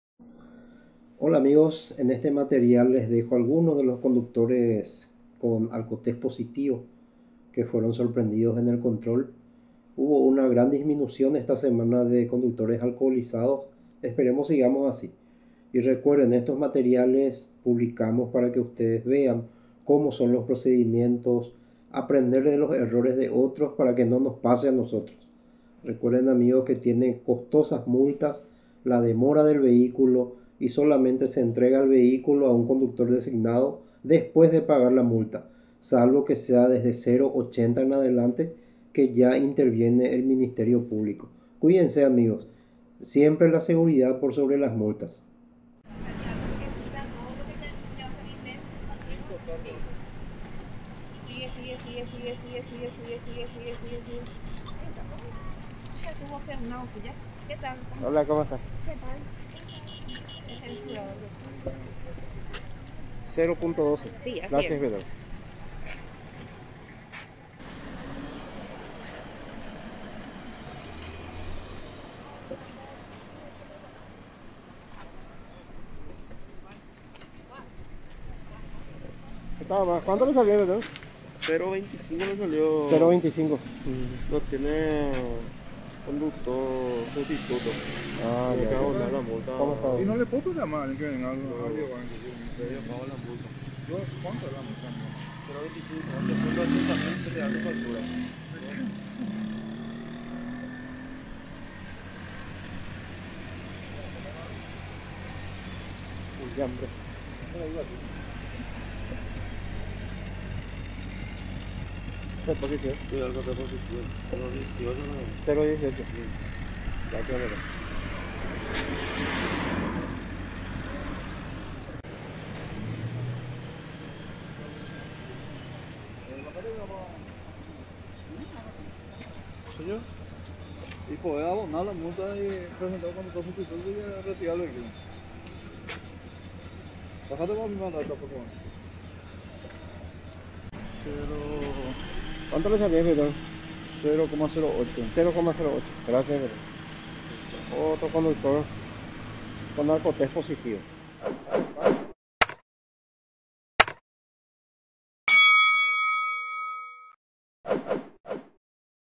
ALGUNOS DE LOS CONDUCTORES QUE DIERON POSITIVO AL ALCOTEST ESTA SEMANA
Cuidense amigos, dejemos las bebidas de lado si vamos a conducír!
Alberto Meza - Desde las Rutas del Paraguay